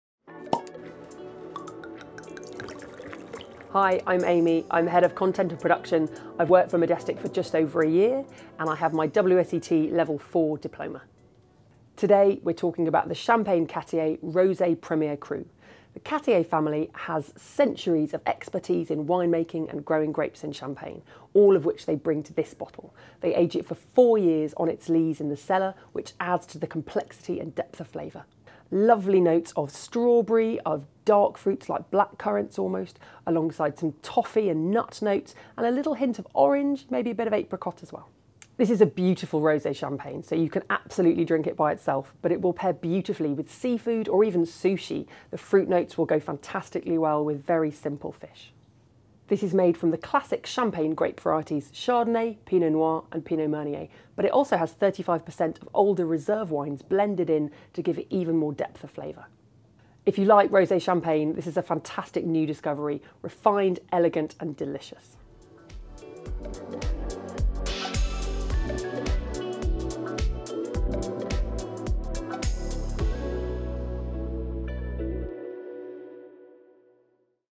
Discover the Cattier Premier Cru Rosé Champagne with Majestic
Discover the Cattier Premier Cru Rosé Champagne with Majestic, the UK's number one specialist wine retailer. A chance to get inside the bottle with members of the Majestic team from all over the country.